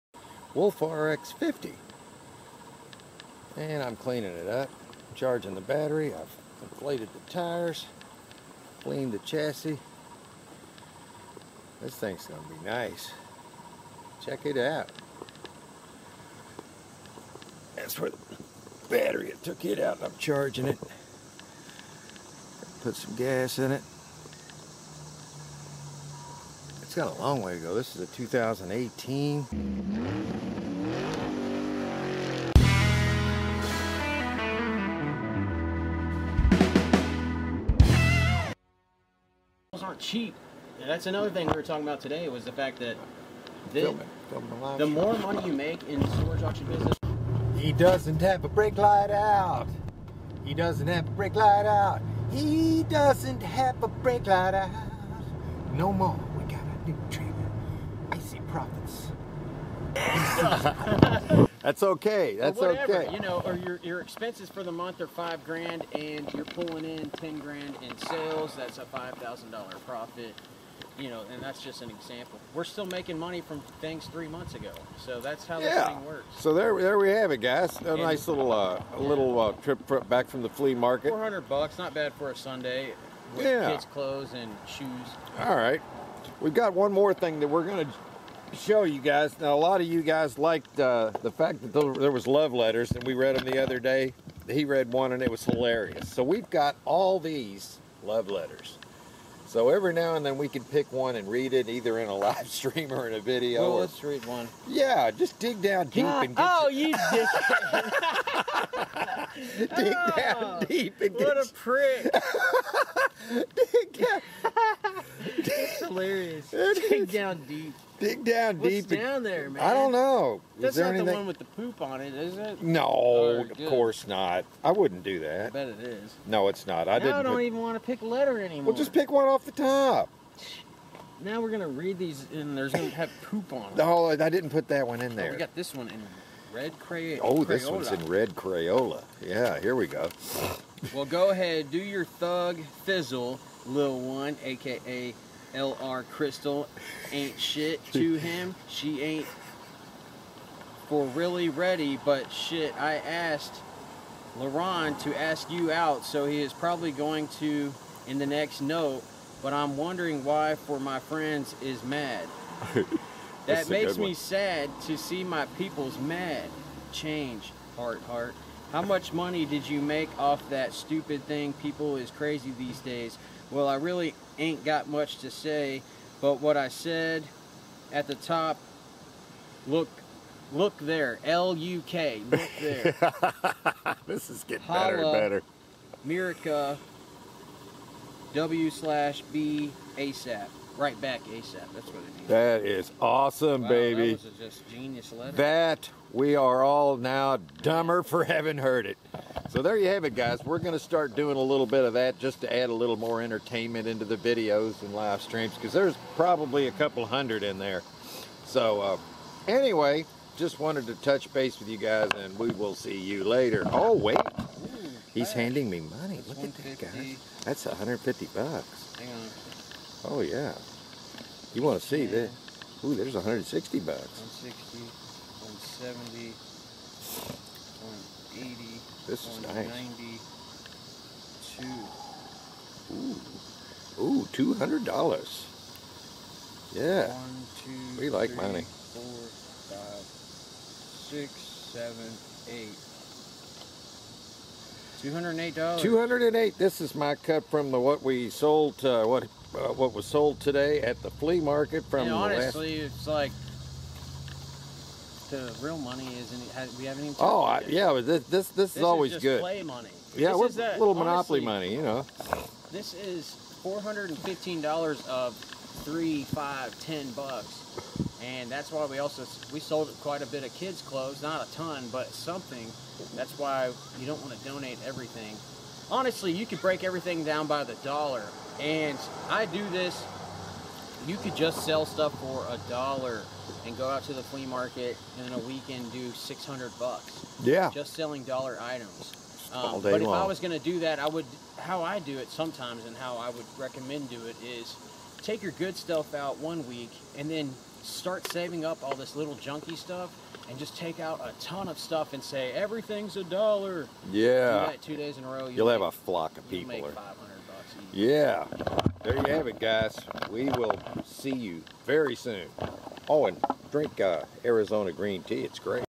Storage Auction Liquidation Tips Reading Locker Love Letter
Icy Profits has some great tips. Reading of storage unit love letter.